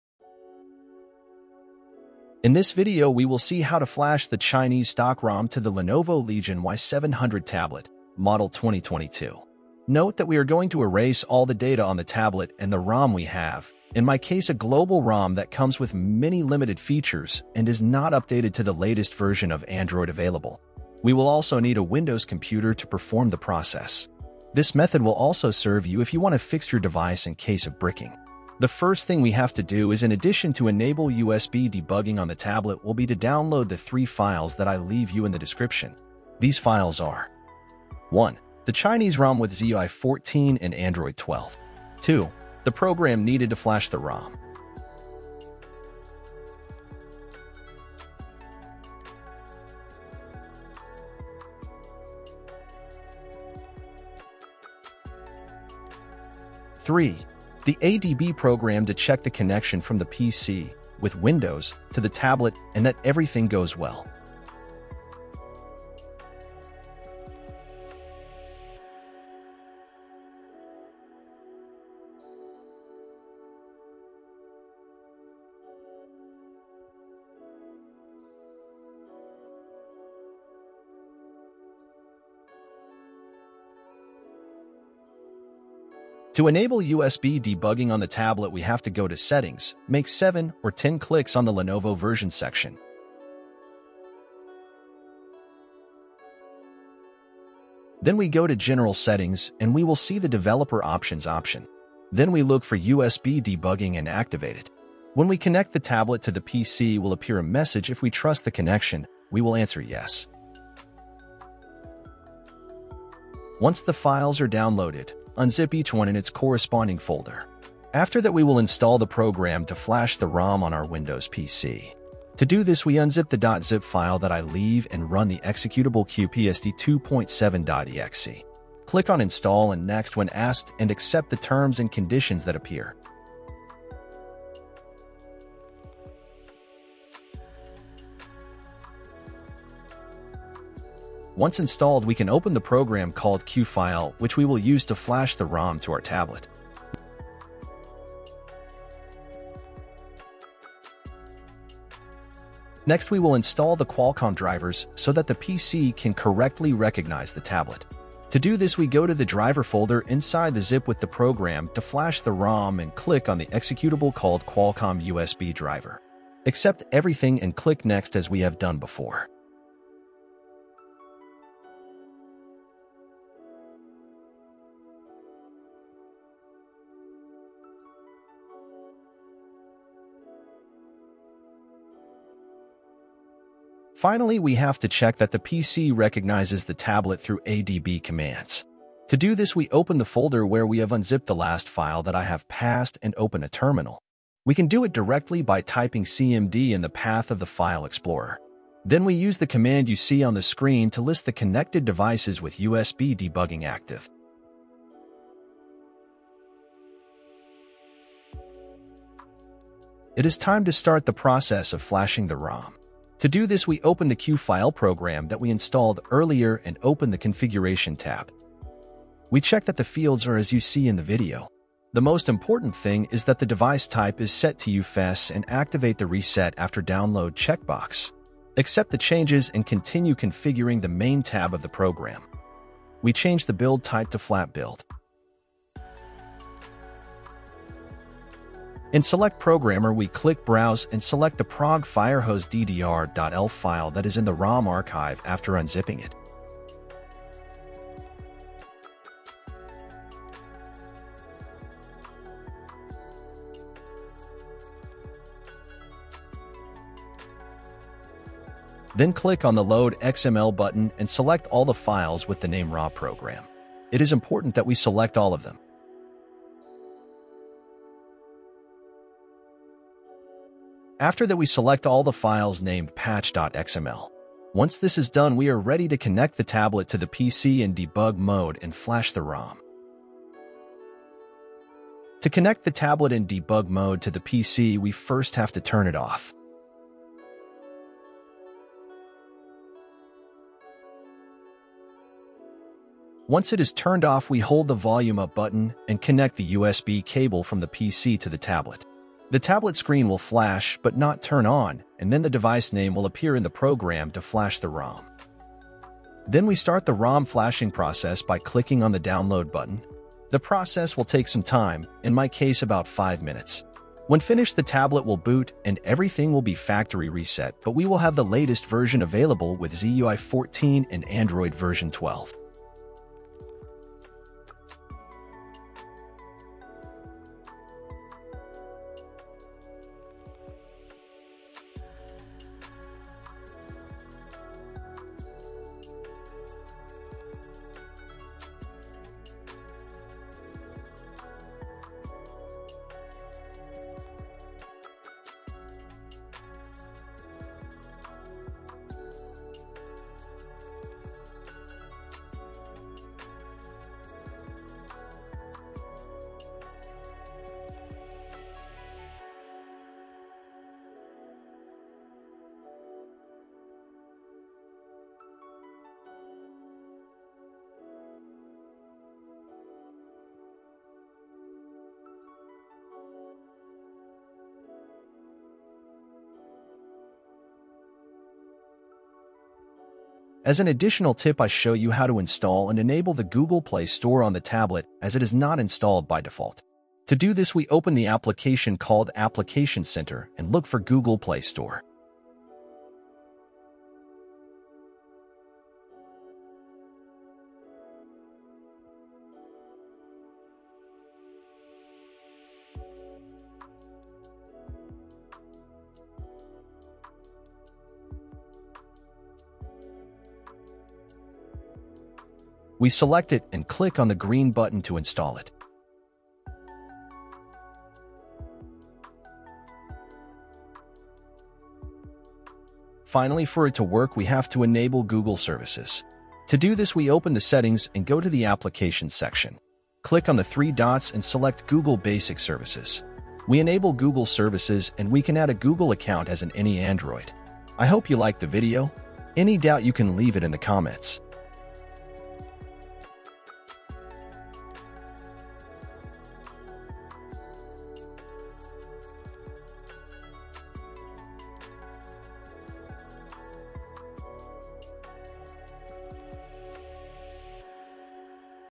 How to flash CN STOCK ROM to Lenovo LEGION Y700 2022 with ZUI14 (TB9707F) and Google Play - 2024
In this video I show you how to flash the Chinese stock ROM to the Android tablet Lenovo Legion Y700 model 2022.

This ROM has ZUI14, is based on Android 12 and has only 2 languages: Chinese and English but allows us to have all the features that has this tablet (which only came out in the Chinese market).

If you want to read more information about the proccess check the post in XDA where I got all the info

⚠️ ⚠️ NEW REPO with more links and all the info about this tablet